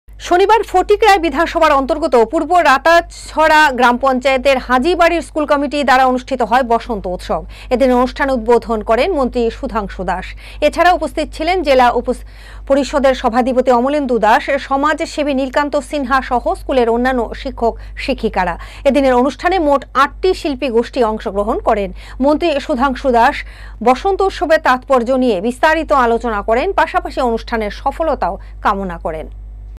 পূর্ব রাতাচড়া গ্রাম পঞ্চায়েতের হাজী বাড়ির স্কুল কমিটি দ্বারা অনুষ্ঠিত হয় বসন্ত উৎসব।
শনিবার ফটিকরায় বিধানসভার অন্তর্গত পূর্ব রাতাচড়া গ্রাম পঞ্চায়েতের হাজী বাড়ির স্কুল কমিটি দ্বারা অনুষ্ঠিত হয় বসন্ত উৎসব। এদিনের অনুষ্ঠানের উদ্ভোধন করেন মন্ত্রী শুধাংসু দাস। এছাড়া উপস্থিত জেলা পরিষদের সভাধিপতি অমলেন্দু দাস, সমাজসেবী নীলকান্ত সিনহা, সহ স্কুলের অন্যান্য শিক্ষক শিক্ষিকারা। এদিনের অনুষ্ঠানে মোট আটটি শিল্পীগোষ্টি অংশগ্রহণ করেন। মন্ত্রী শুধাংসু দাস বসন্ত উৎসবের তাৎপর্য নিয়ে বিস্তারিত আলোচনা করেন পাশাপাশি অনুষ্ঠানের সফলতা কামনা করেন।